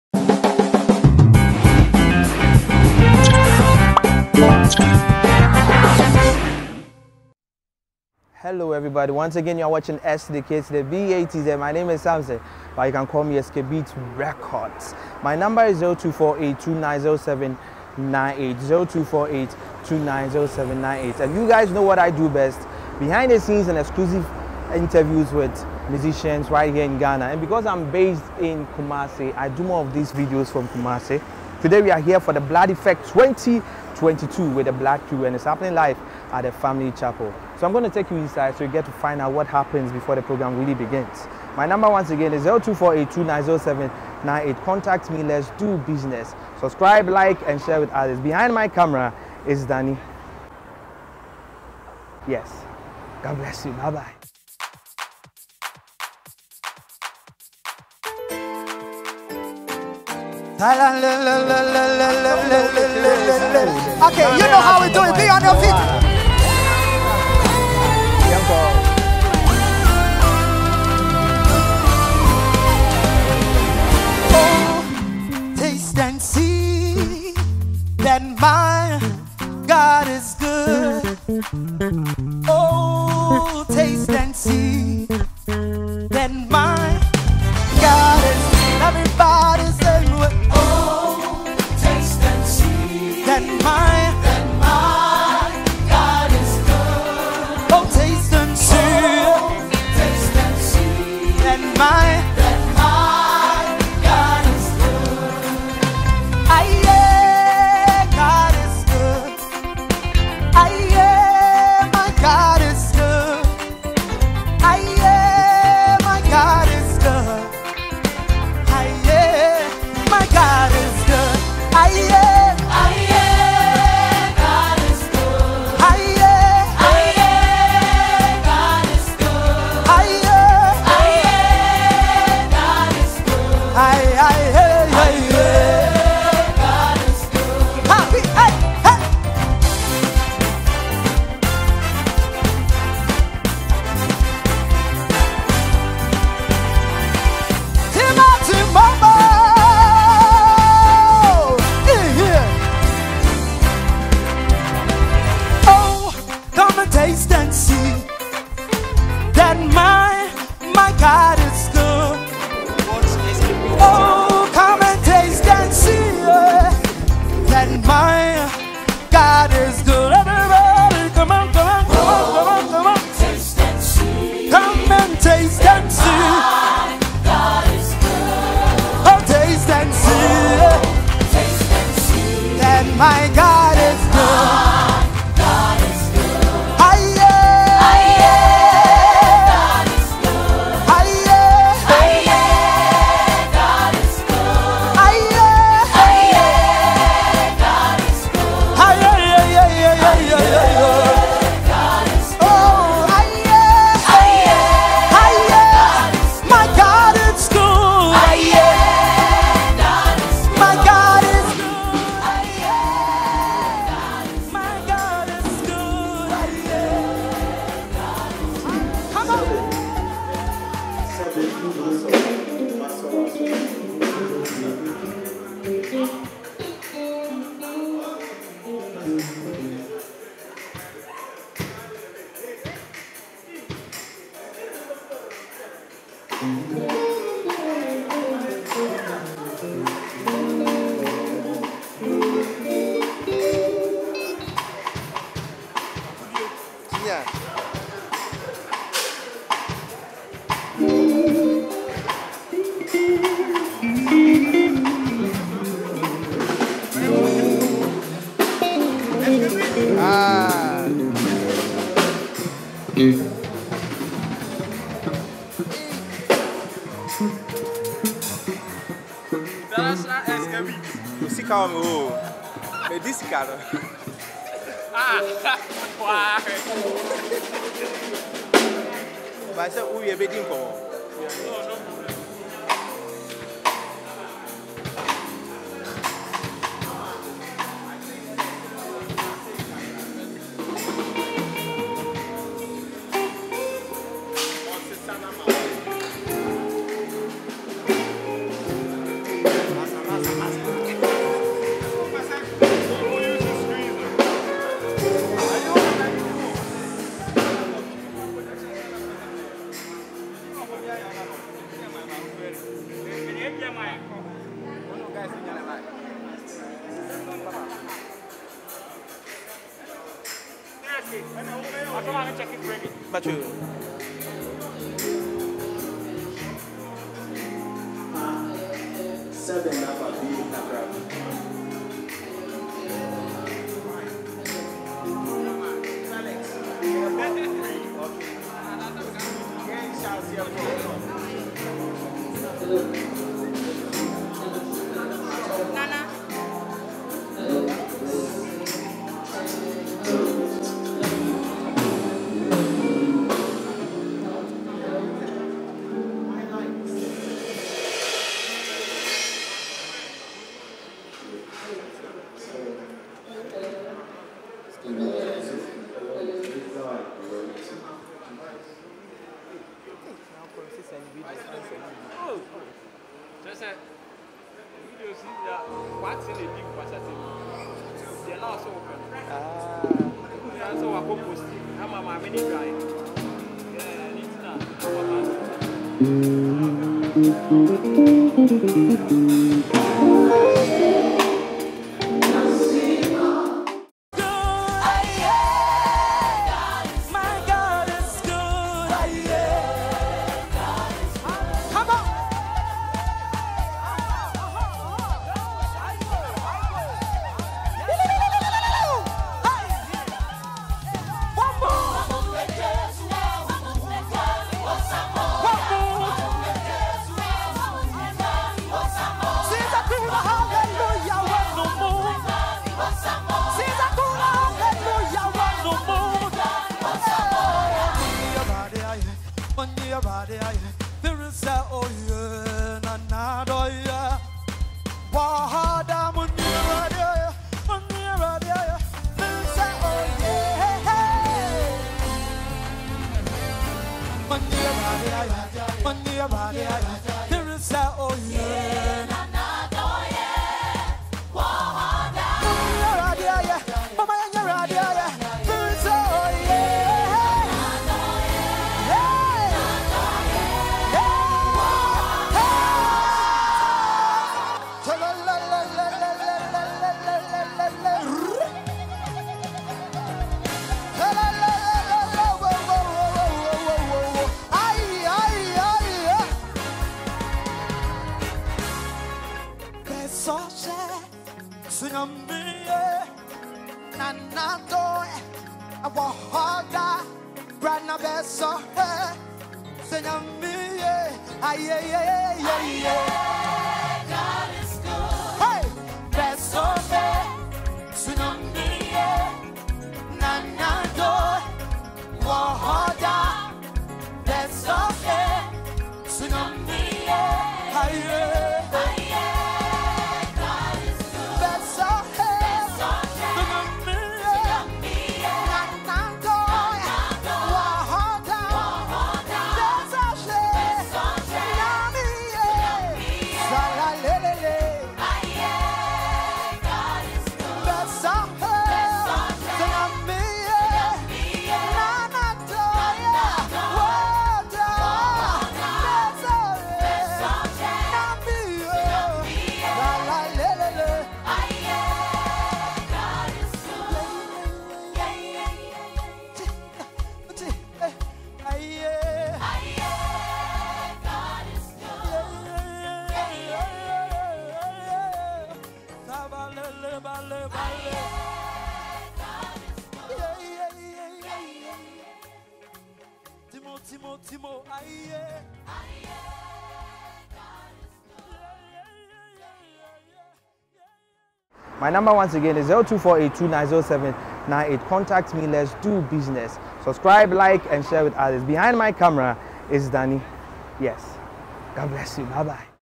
The BLOOD EFFECTS 2022 | Behind The Scenes | Dress Rehearsal + Set Up || The BLOOD Crew|| Skbeatz
For more of these Exclusive Interviews and contents;